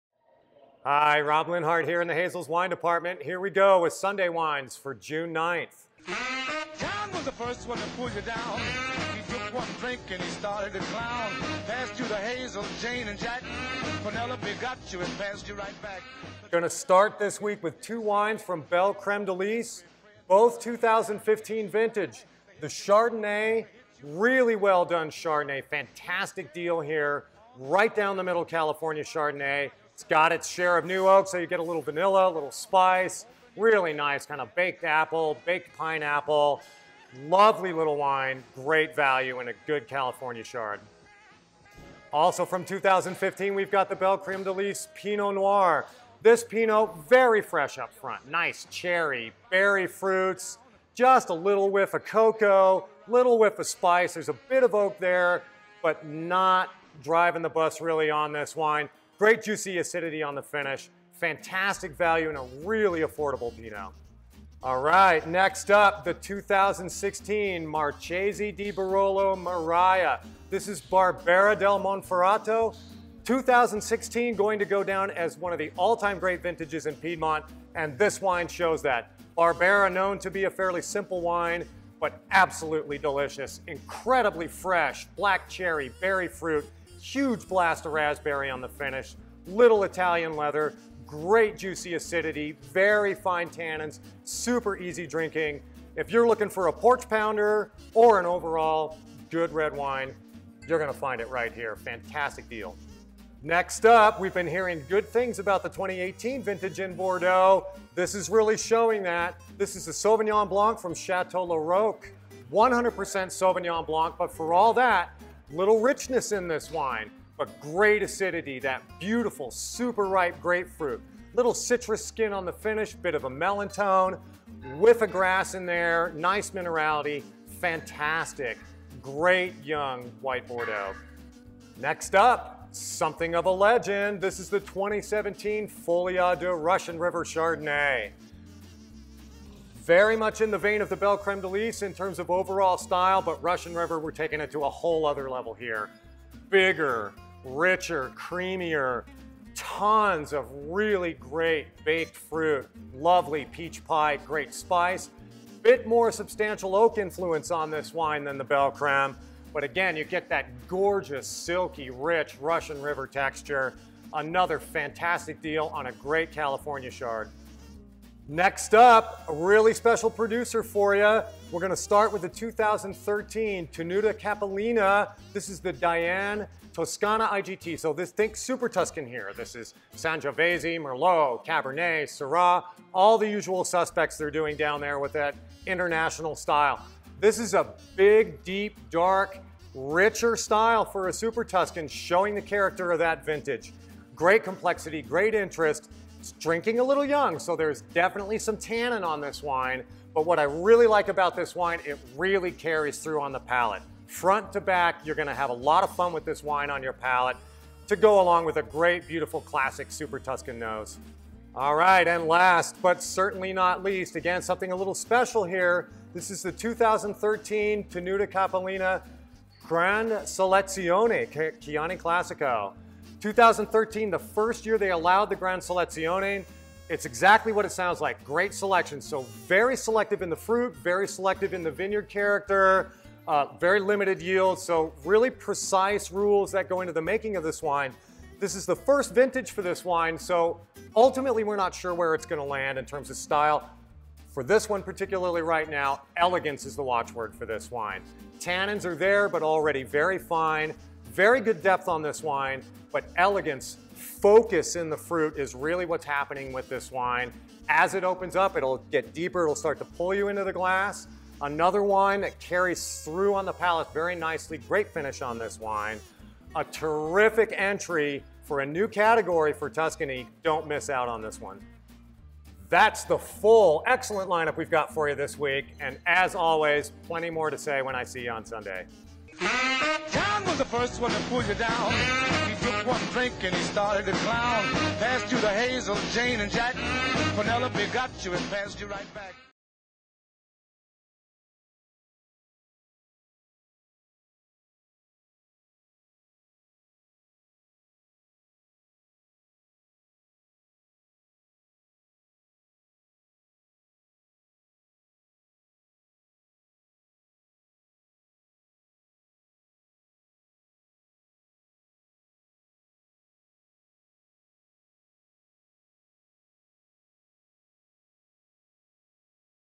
A Grand Selection with a Gran Selezione
A very special Chianti Classico is featured in this weekend's Zunday Zpecial at Hazel's Beverage World.  And that's just one of seven great wines.  Want to know what's in the bottle before you buy?  Check out Rob's thorough tasting notes in this video, then get in to Hazel's this Sunday for amazing prices on these wines and more!